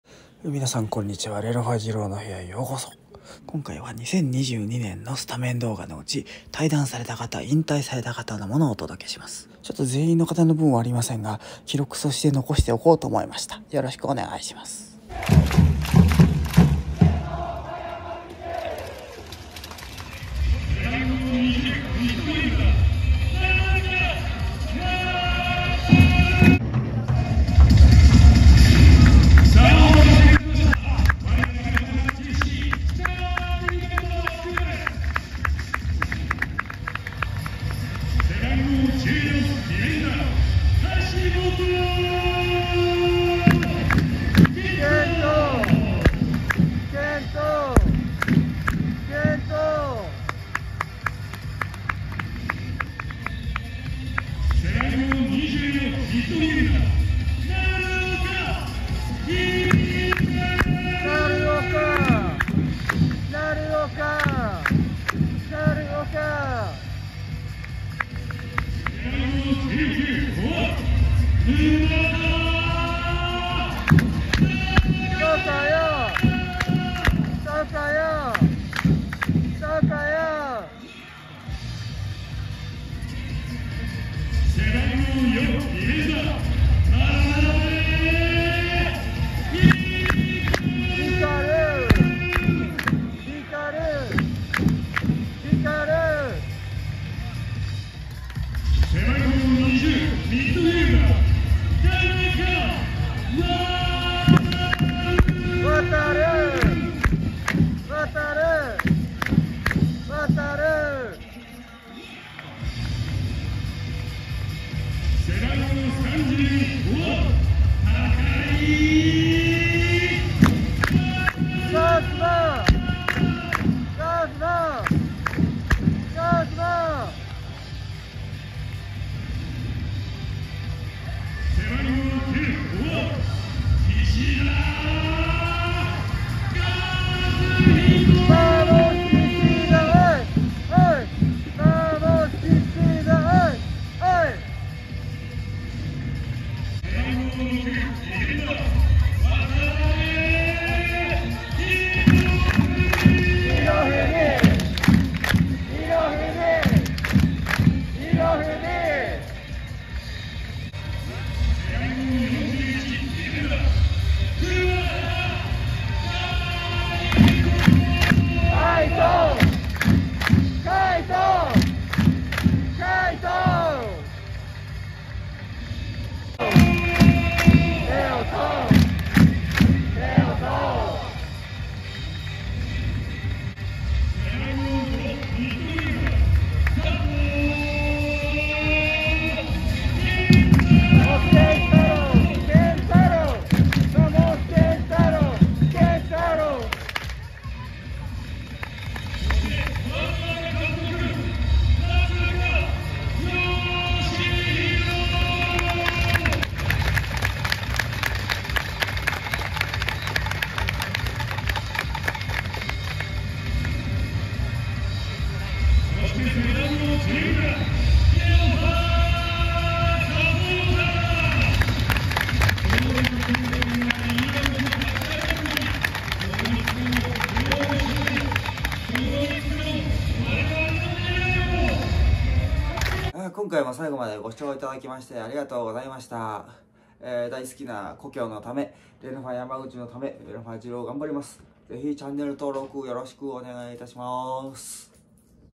【2022年】レノファ山口スタメン紹介 退団・引退選手
2022年のレノファ山口のスタメン紹介の動画が保存されていて
せっかくなので記録用に残す事にしました。
渡部博文選手（現社長）や橋本健人選手（横浜FC）。。
退団・引退した選手分を集めました。
一部選手はありませんが振り返って楽しんでいただければと思います。

引き続き頑張れレノファ山口！

地元のＪ２チーム、レノファ山口を楽しく熱く応援するチャンネルです。動画を通じて一人でも多くの人がファンになってくださると嬉しいです。

▼もくじ▼
0:00 オープニング
0:38 橋本健人選手（横浜FC）
0:54 成岡輝瑠選手（清水）
1:10 沼田駿也選手（町田）
1:25 眞鍋旭輝選手（宮崎）
1:40 田中渉選手（山形）
1:55 高井和馬選手（横浜FC）
2:12 岸田和人選手（下関）
2:27 渡部博文選手（現社長）
2:42 桑原海人選手（鈴鹿）
2:57 兒玉澪王斗選手（鈴鹿）
3:07 佐藤健太郎選手（現強化部）
3:21 名塚善寛監督